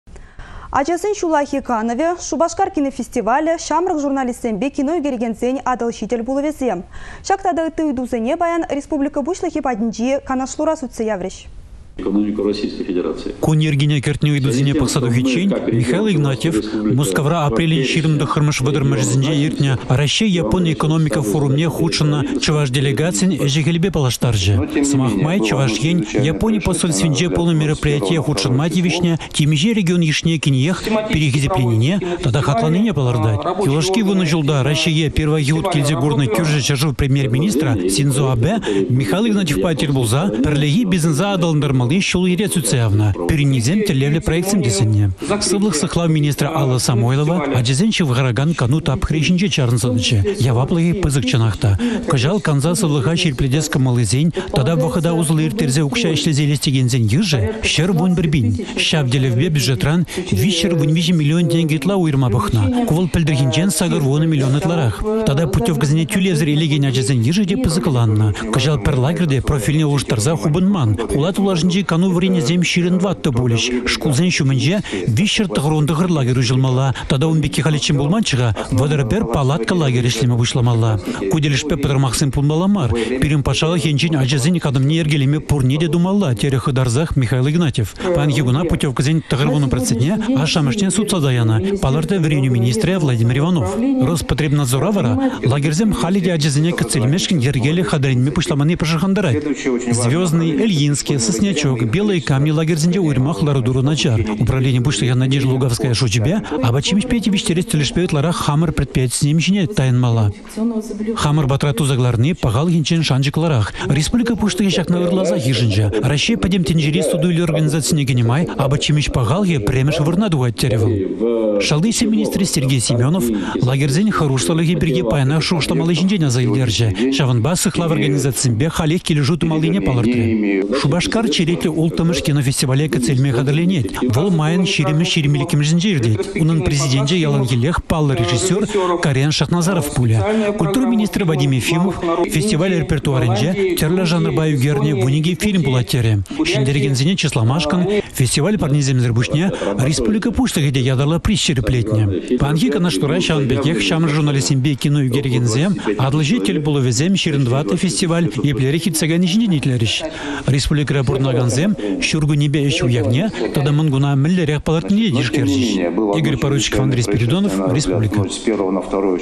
Çивĕч ыйтусене республика пуçлăхĕ патĕнче сÿтсе яврĕç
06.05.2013
Ачасен çуллахи канăвĕ, Шупашкар кинофестивалĕ, çамрăк журналистсемпе кино ÿкерекенсен «Атăлçи тĕл пулăвĕсем». Çак тата ытти ыйтусене паян республика пуçлăхĕ патĕнче канашлура сÿтсе яврĕç.